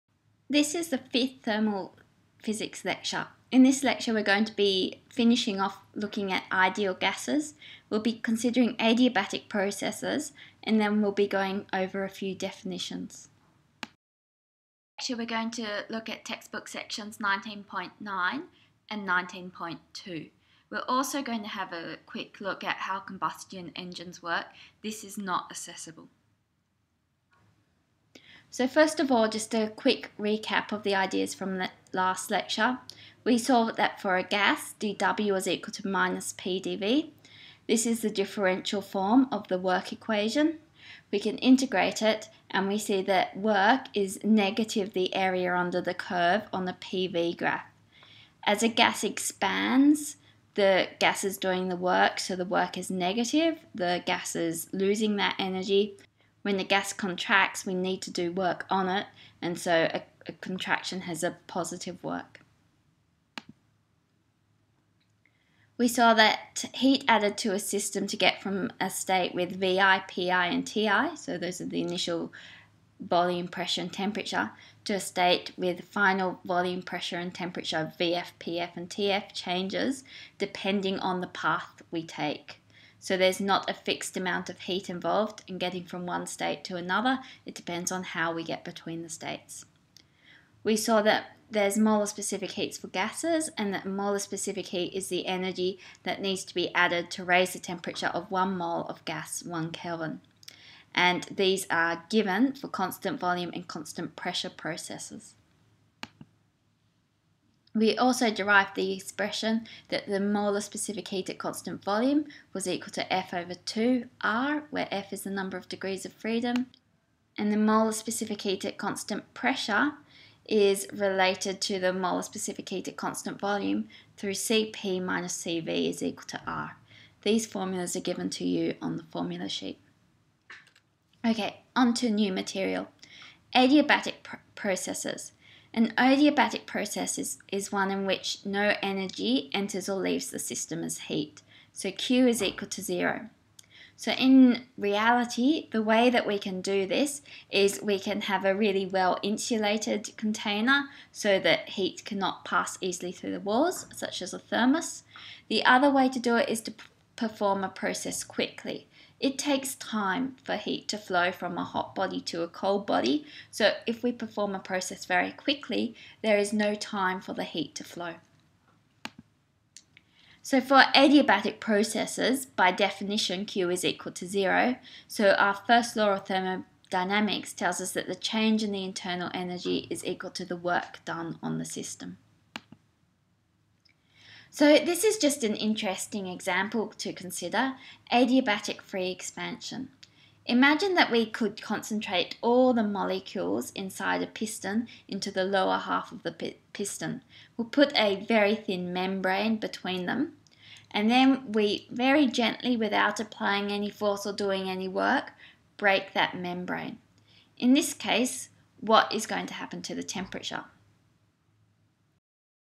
Introduction thermal 5
This video introduces you to the fifth thermal physics lecture and defines adiabatic processes.